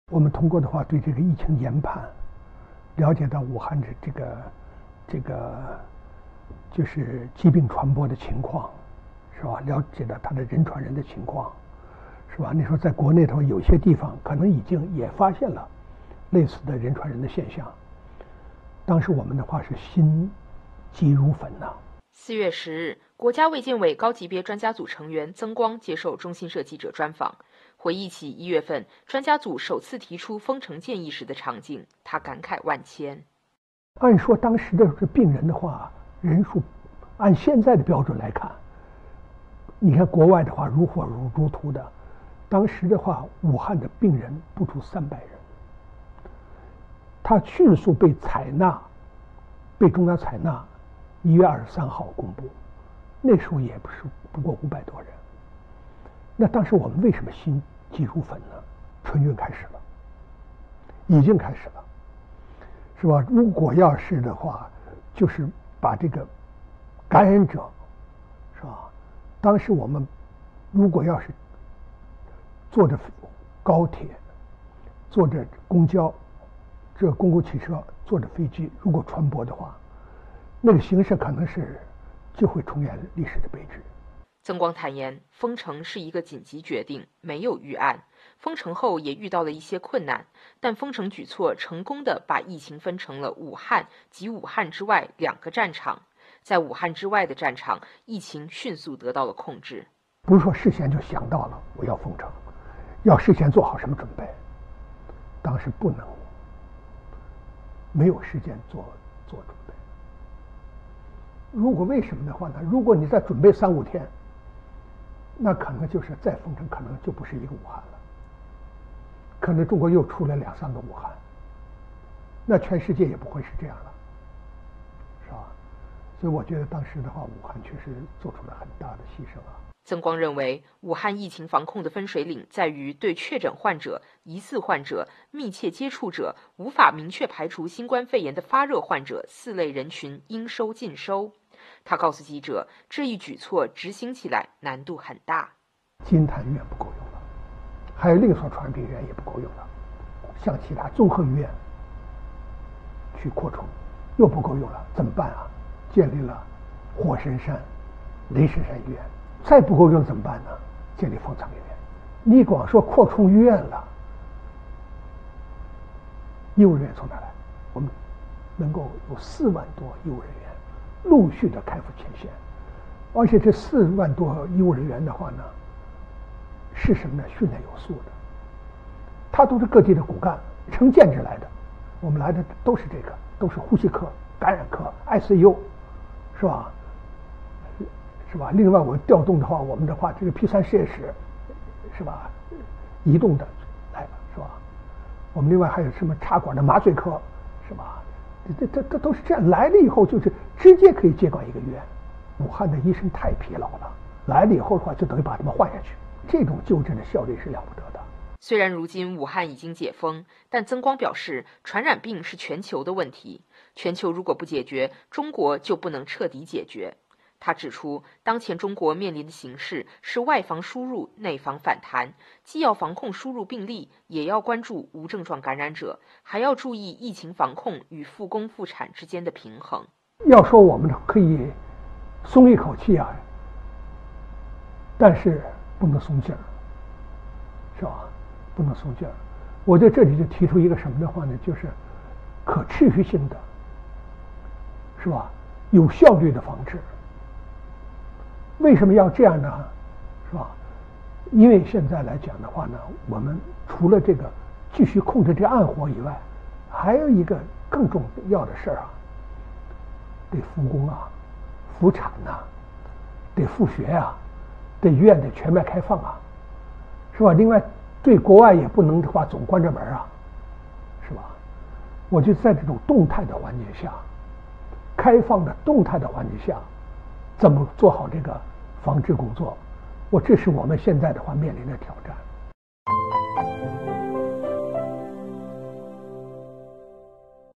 曾光忆述专家组建议武汉“封城”：人传人现象让我们心急如焚
4月10日，国家卫健委高级别专家组成员曾光接受中新社记者专访，回忆起1月份，专家组首次提出封城建议时的场景，他感慨万千。曾光坦言，“封城”是一个紧急决定，没有预案。但“封城”举措成功地把疫情分成了武汉及武汉之外两个“战场”。在武汉之外的“战场”，疫情迅速得到了控制。曾光认为，虽然如今武汉已经解封，但传染病是全球的问题，全球如果不解决，中国就不能彻底解决。